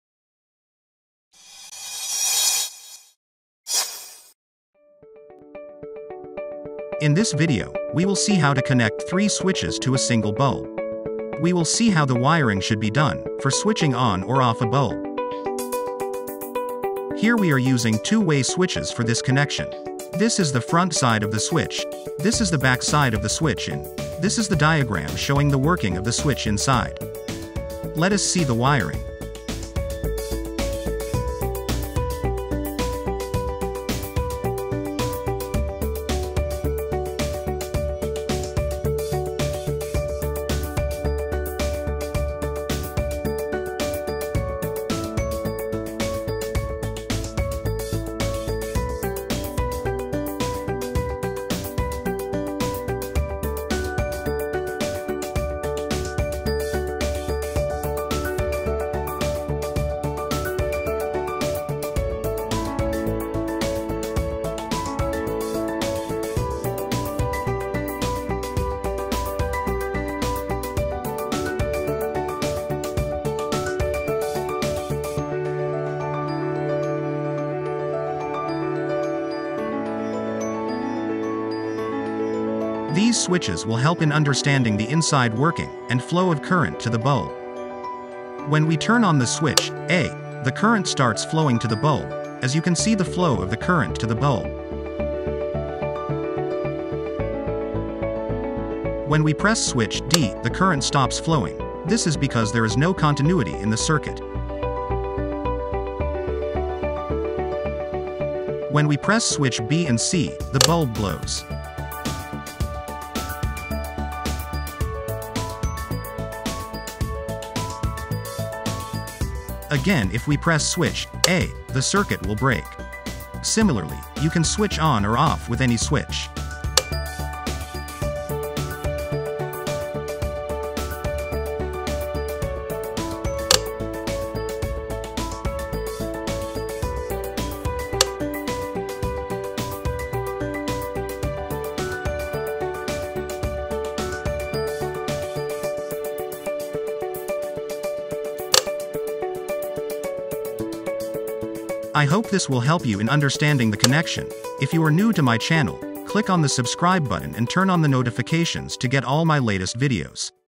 1 Bulb with 3 Switches Connection Diagram | Electric connection to the bulb
This video shows how to connect a single bulb to 3 switches.

1 bulb 3 switch connection,
how to connect a single bulb with three switches,
electric connection to the bulb from 3 switches,